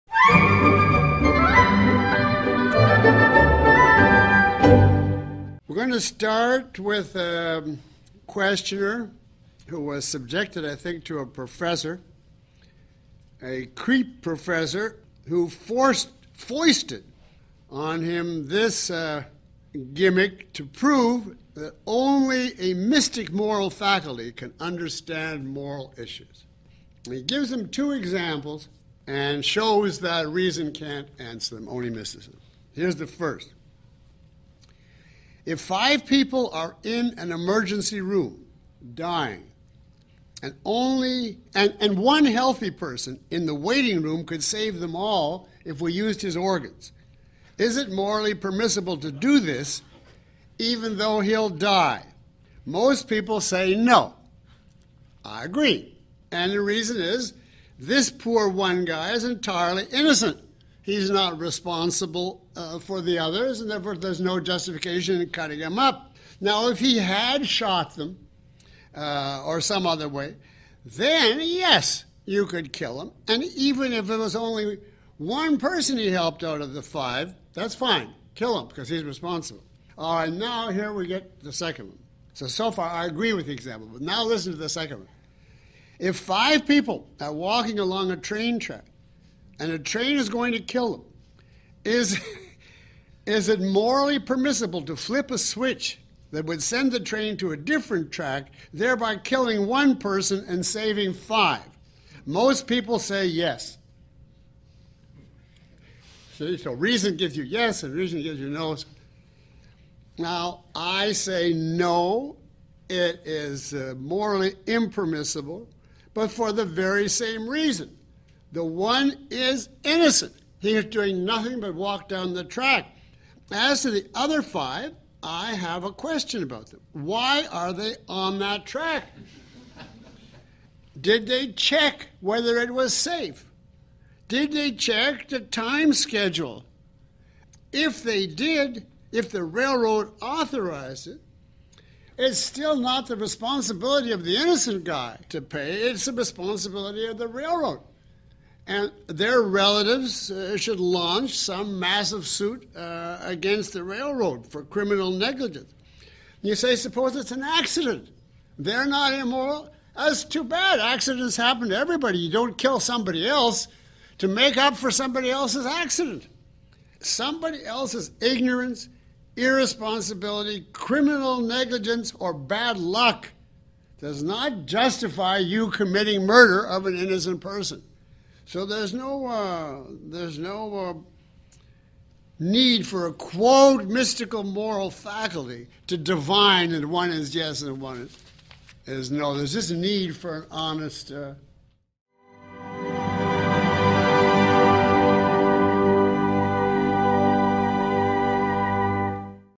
El dilema moral de la vía del tren
Las situaciones de "emergencia" que la gente se inventa pueden ser complicadas, y en muchos casos la ética no tiene reglas para lidiar con ellas; pero cuando las tiene, nunca tiene que apelar a ninguna ética mística. Usar honestamente la mente de uno es suficiente.